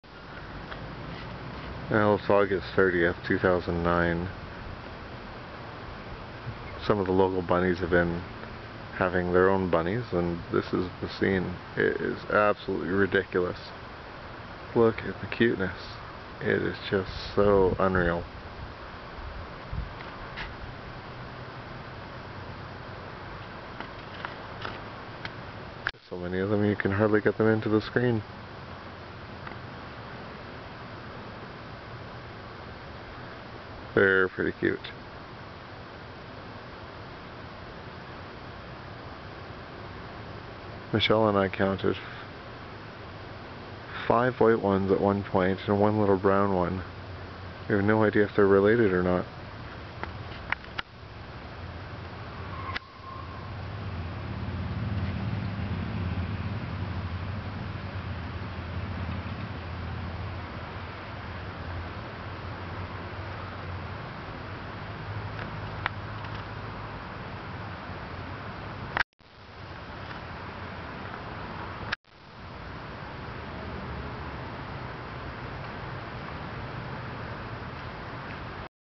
Six baby domestic bunnies found living in our backyard
On August 29, 2009, we discoverd that baby bunnies were living underneath our wooden shed.  Originally, we thought that there were only three babies, but have discovered that there's now a total of six - 5 white with brown markings, and one chesnut brown one.  The parents come by every now and then to check on their babies.